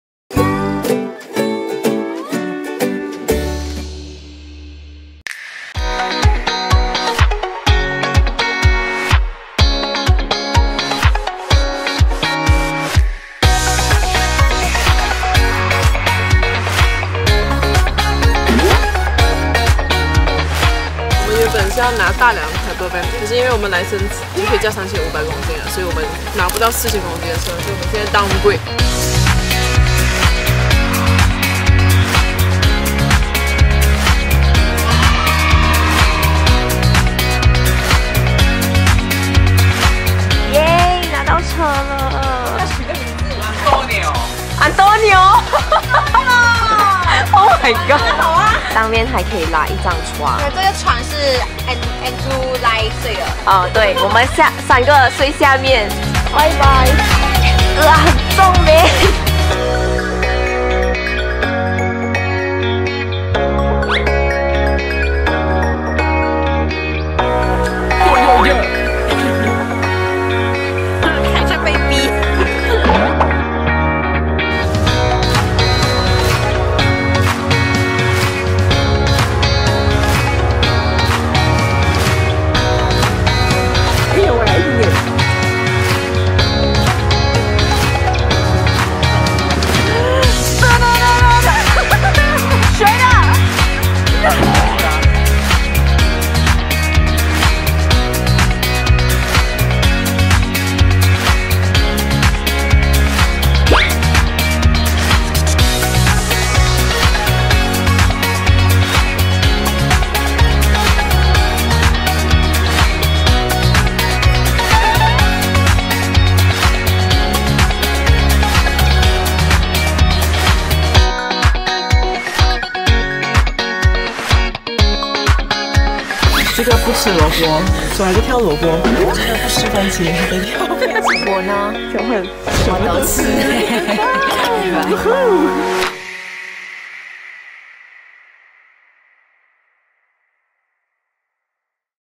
Travel Tasmania with a caravan | 塔斯馬尼亞自由行 | 房車自由行
塔斯馬尼亞, 一座漂亮的心型小島; 圍繞著山脈, 平原與小丘. 與大自然為伍, 與溫柔的人兒相伴; 看日出日落, 逛小城鎮, 爬小山坡, 夕下後就開始數星星的節奏, 以及深夜的心靈對談. 每個轉彎都充滿著驚喜, 不小心就把自己的一部分遺留在小島上, 真心喜歡與自然共舞的感覺.